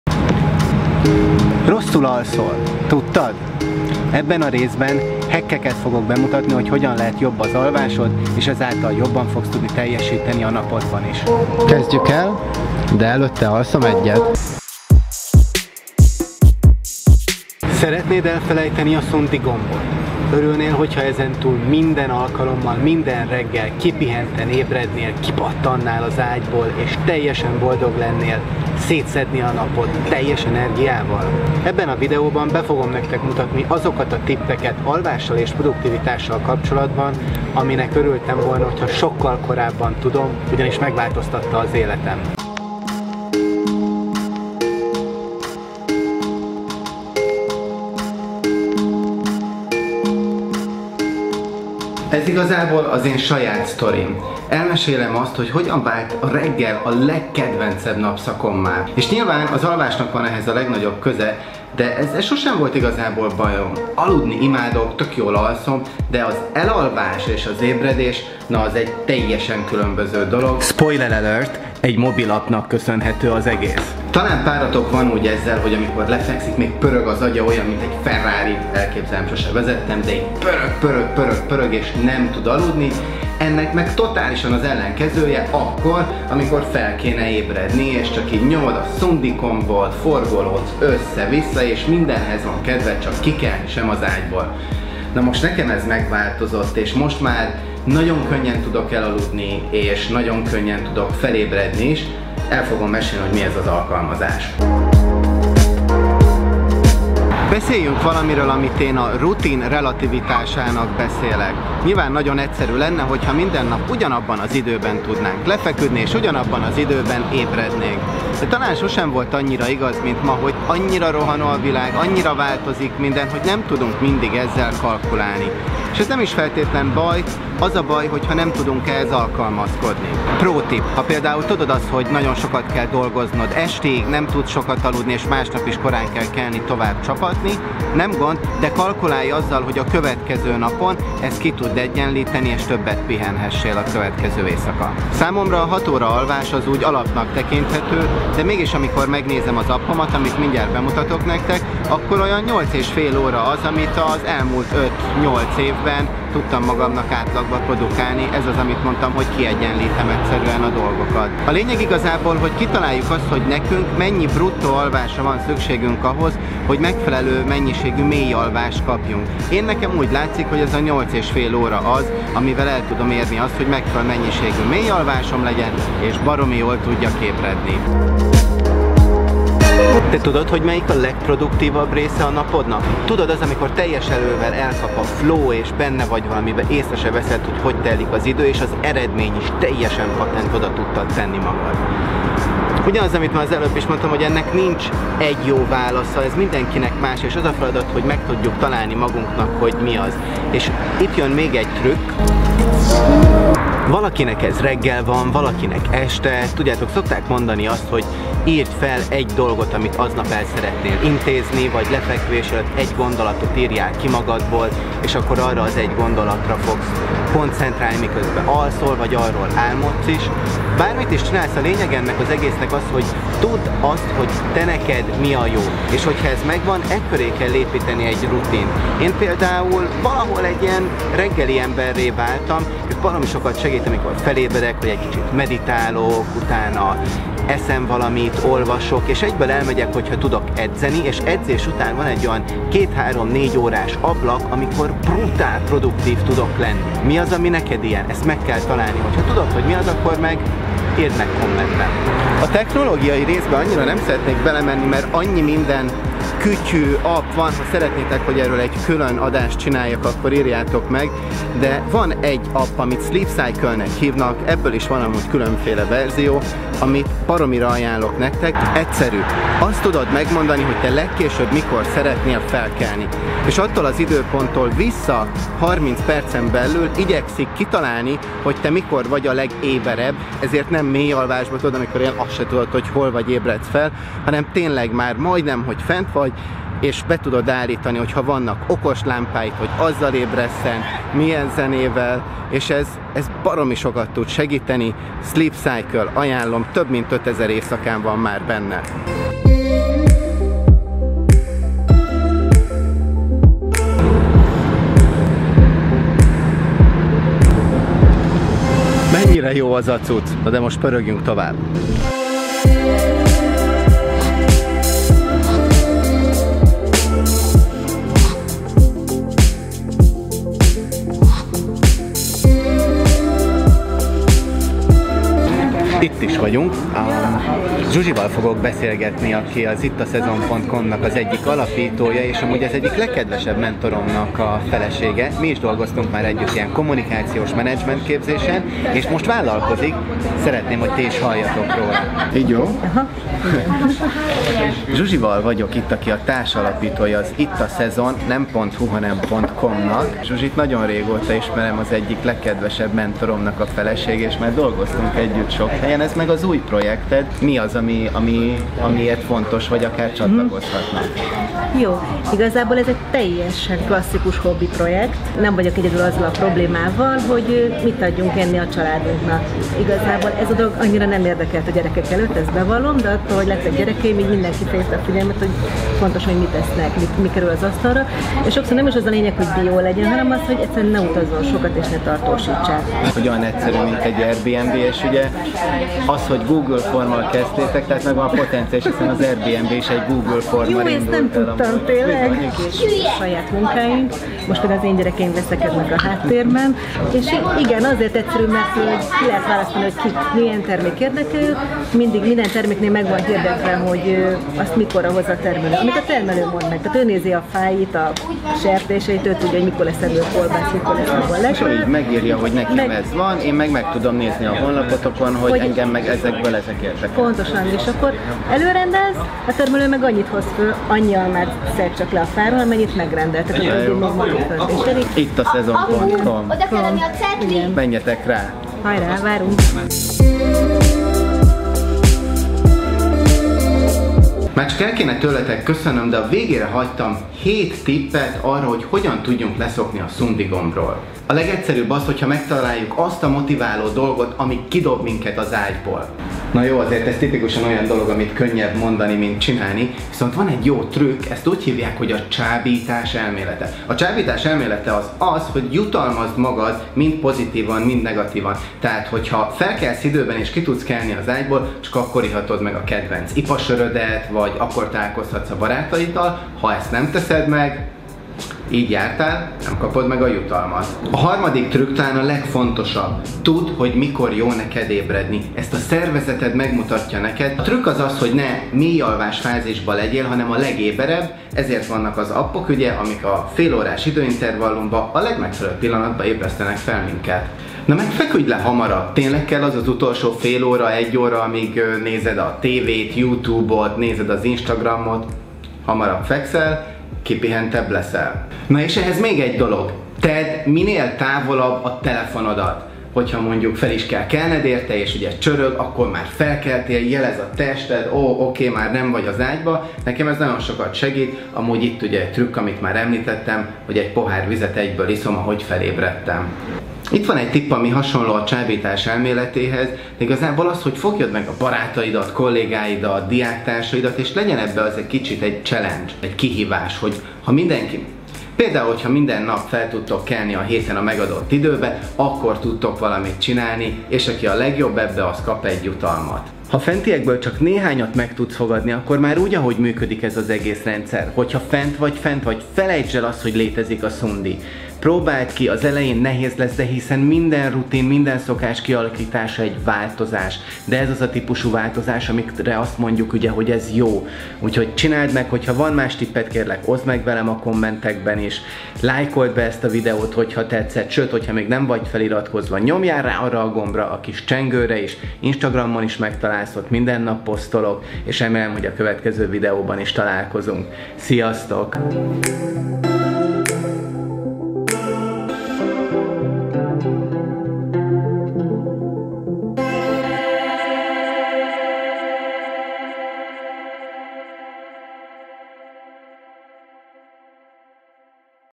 7 TRÜKK, HOGY A SZUNDI GOMBOT SOSE NYOMD LE TÖBBÉ ✖️Hunbelievable #55 ✖️SZÁNTÓ PÉTER
Rosszul alszol! Tudtad? A videóban bemutatok több módot, amivel meghackelheted az alvásod, hogy 10x produktívabb legyél.

Szó lesz egy mobil appról, ami megváltoztatta az életem. Trükkökről, hogy sose kelljen a szundi gombot lenyomni. És off topic, de bemutatok egy szuper kezdeményezést is

Hogyan szokjunk le a szundi gombról?
1. Ülje le magaddal beszégletni és találd meg azt a csábító indokot, amiért egyből fel fogsz kelni.
2. Jutalmazd magad (és büntesd is): csak akkor vehetsz magadnak egy jó kézművés kávét, ha időben keltél fel. Ezt a csábítás trükkjének nevezik és remekül működik.
3. Ébredj akkora amikor a szervezeted akar, tehát nem mély alvási fázisban. Ehhez használj appokat
4. Feküdj le hamarabb az előző este
5. Tedd távol a telefonod, legyen nehéz benyomni a szundit! Például fel kelljen kellned, elmenni egy másik szobába, stb.
6. Kihívás. Csinálj játékot belőle akár más barátokkal is és legyen egy jó kis tétje.
7. Ha a fentiek működnek már, akkor a felkelésről alkotott képed is meg tudod változtatni: egy döntés. Ha fent vagy, akkor fent vagy és nincs szundi.

Zene: Dyalla